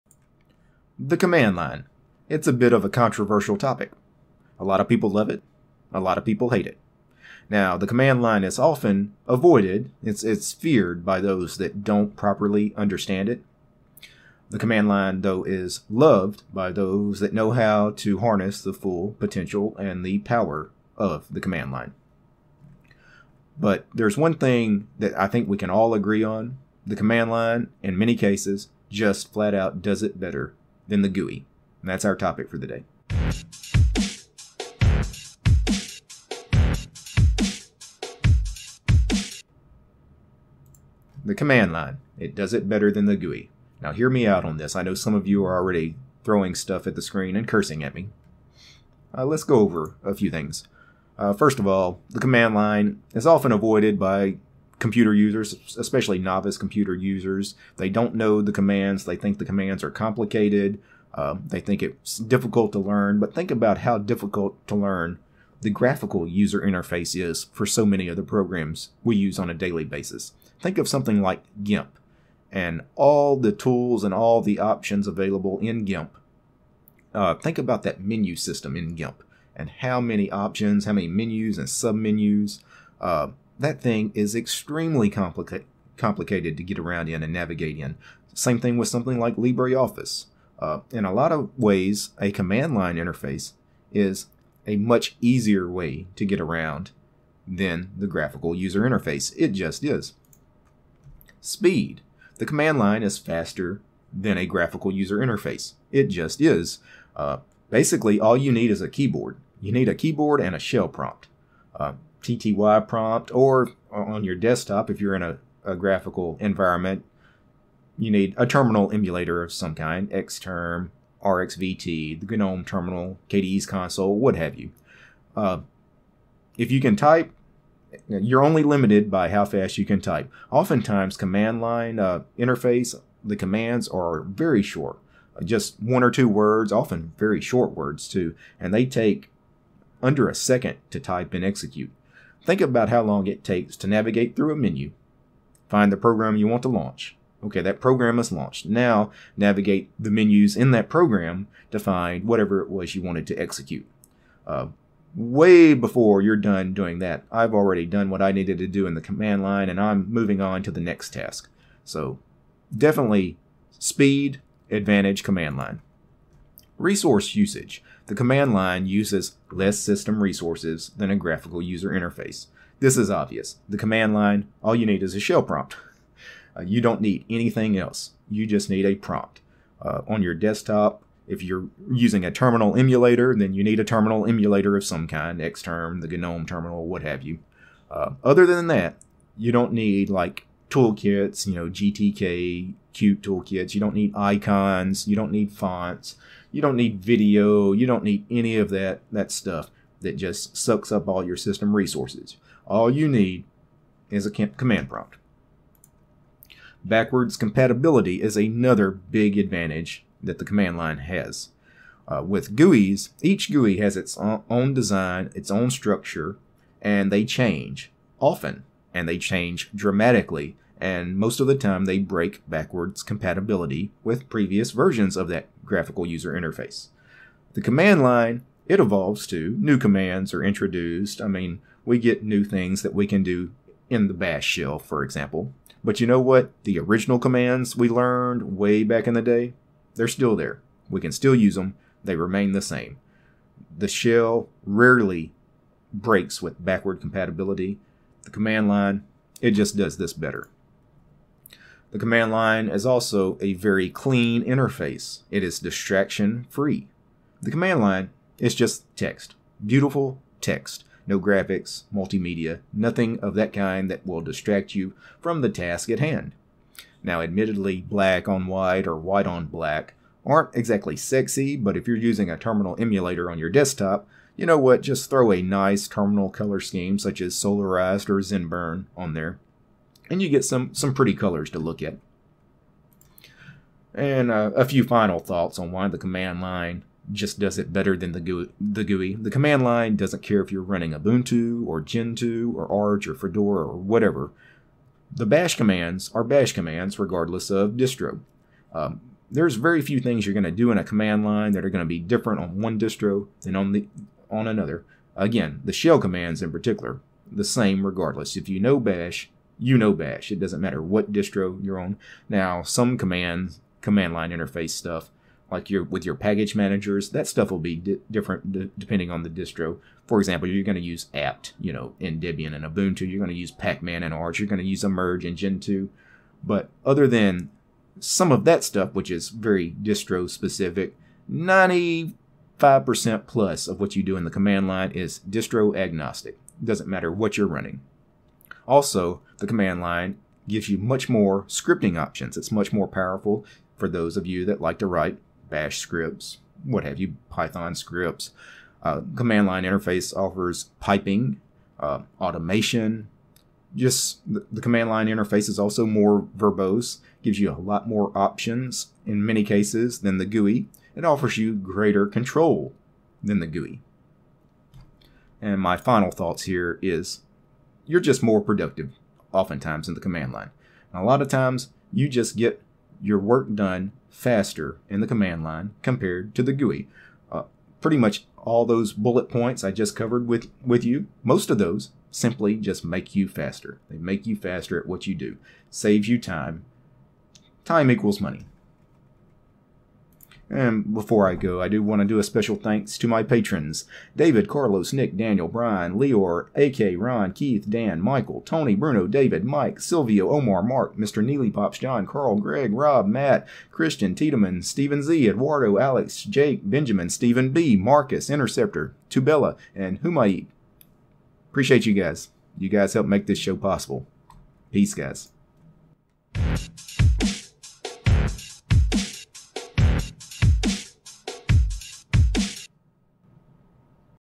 The Command Line Does It Better Than The GUI
We fear what we don't understand.  The command line is no exception.  But with knowledge comes great power!

JOIN ME ON DIASPORA: AND MASTODON